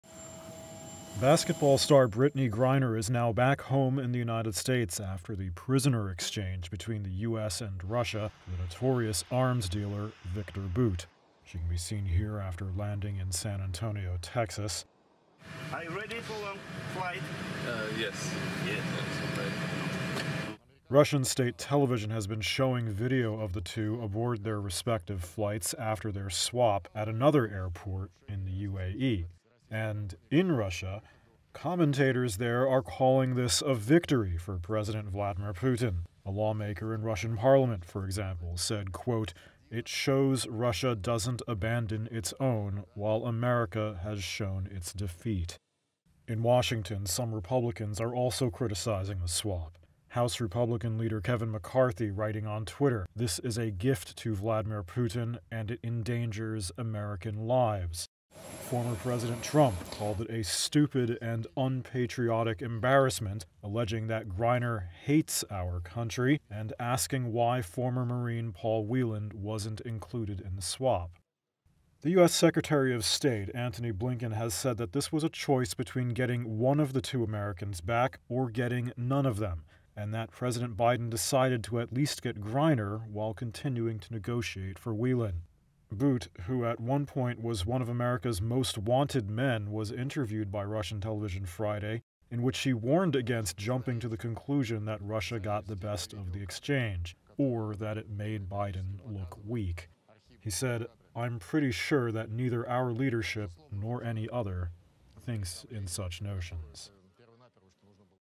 Is the Griner-Bout prisoner swap a Putin victory?
In Russia, commentators are calling the prisoner exchange of U.S. basketball player Brittney Griner for notorious arms dealer Viktor Bout a victory for President Vladimir Putin.